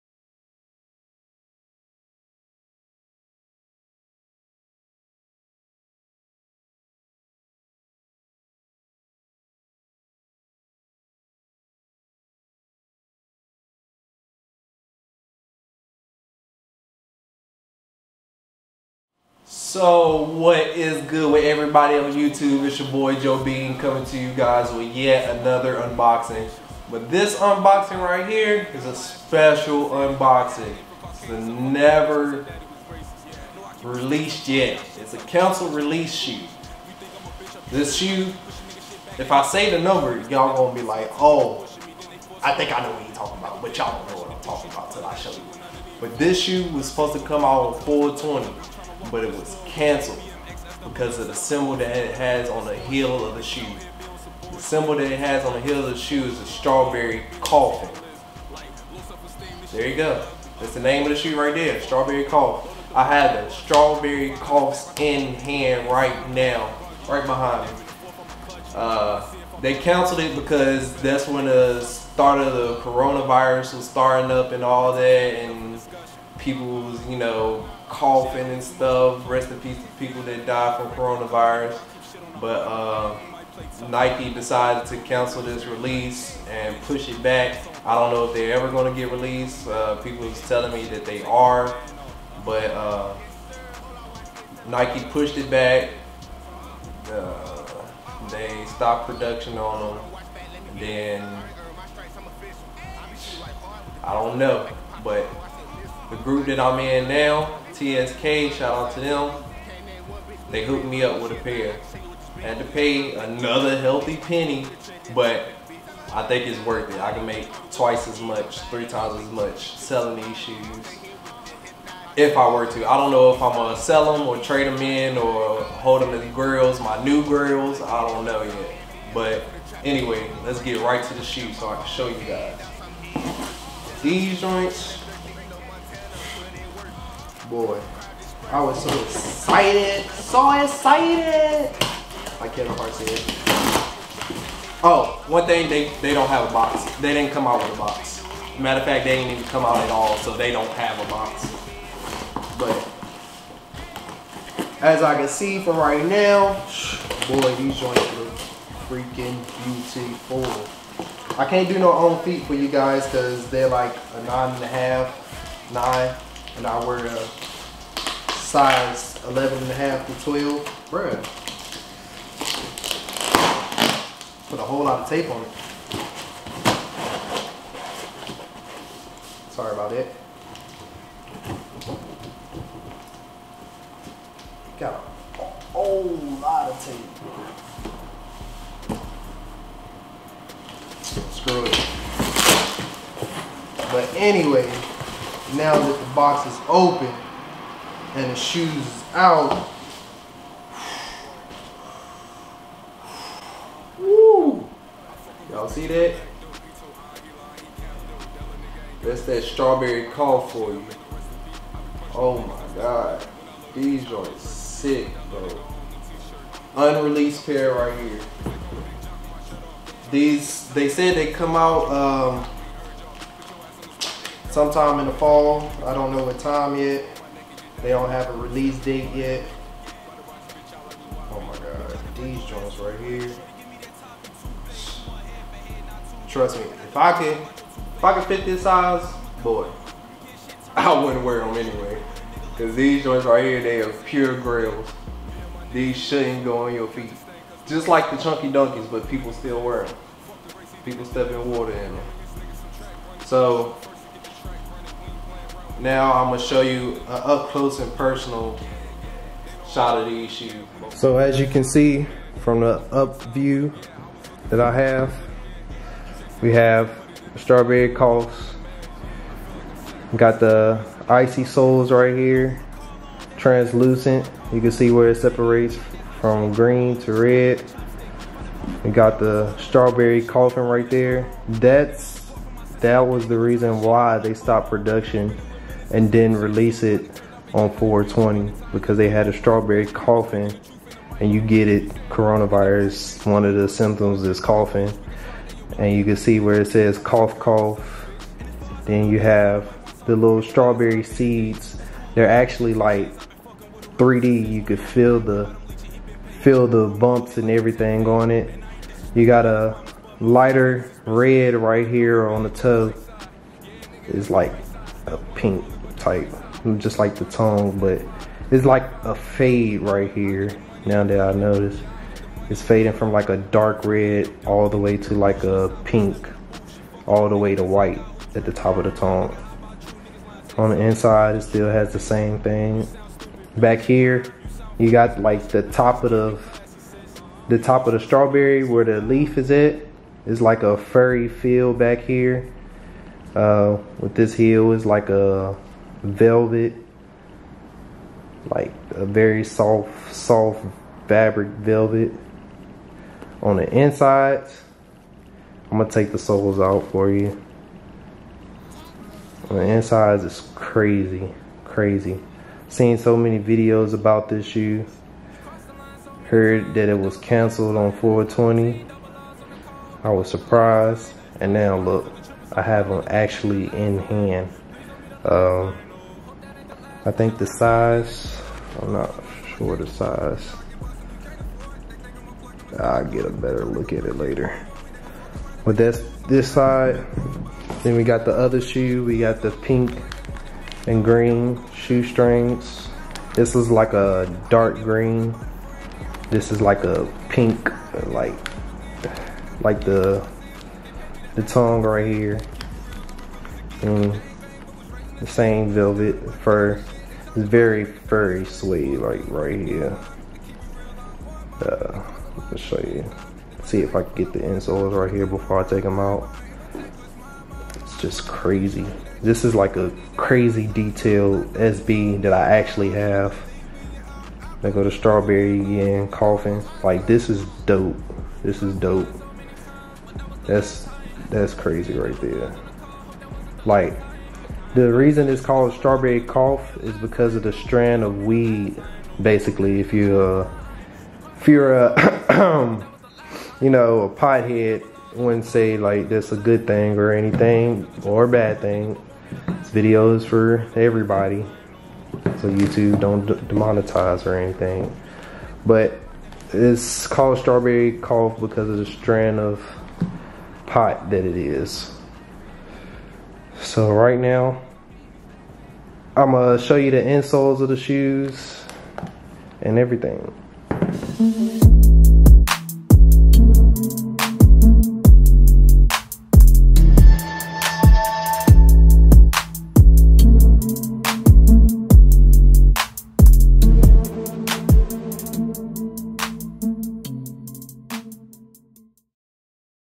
Unreleased Strawberry Coughs
I was able to get a pair of Strawberry Coughs way before their expected release date